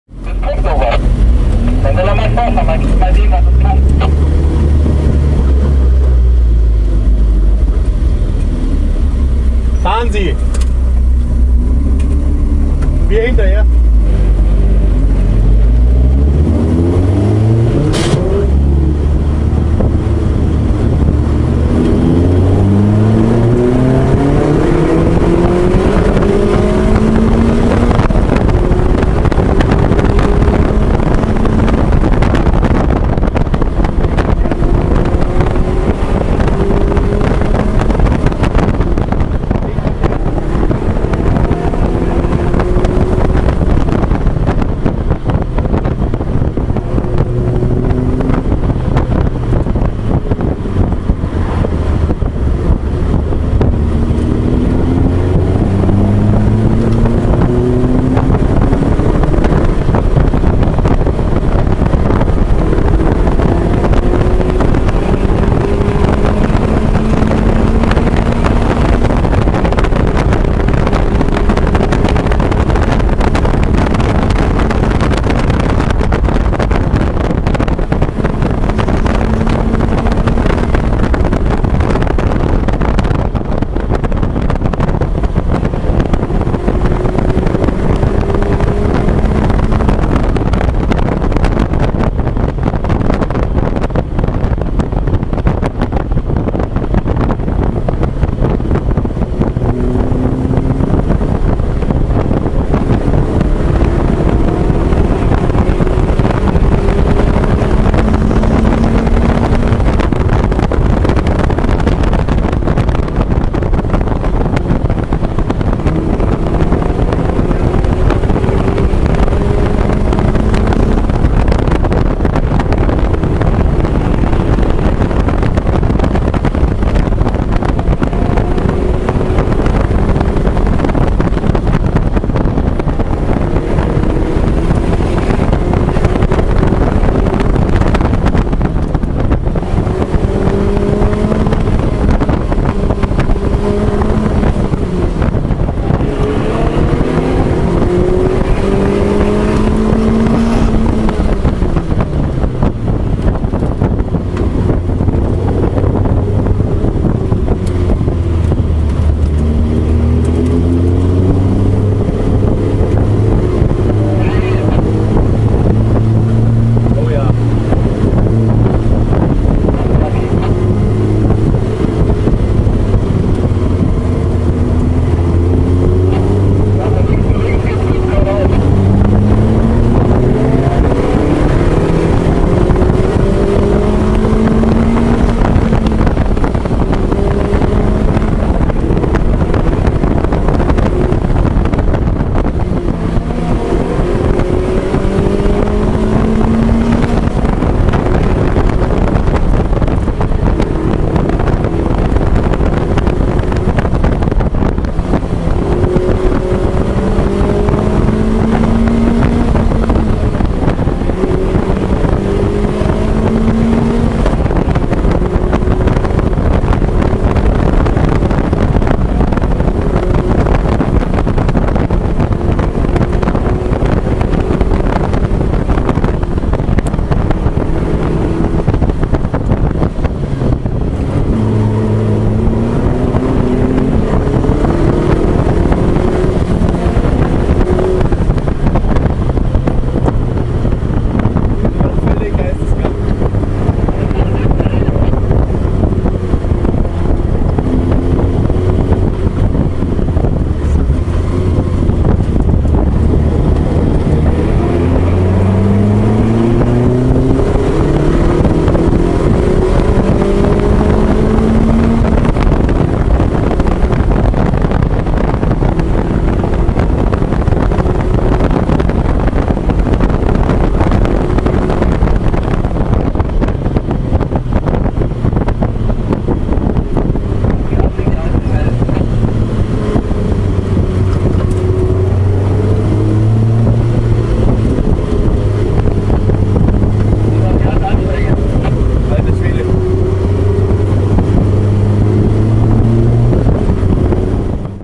Exige V6 having fun with two mad S1 Elises
V6 stock - the S1s... not so much. :D
Huge fun!

Long video of the trip coming soon!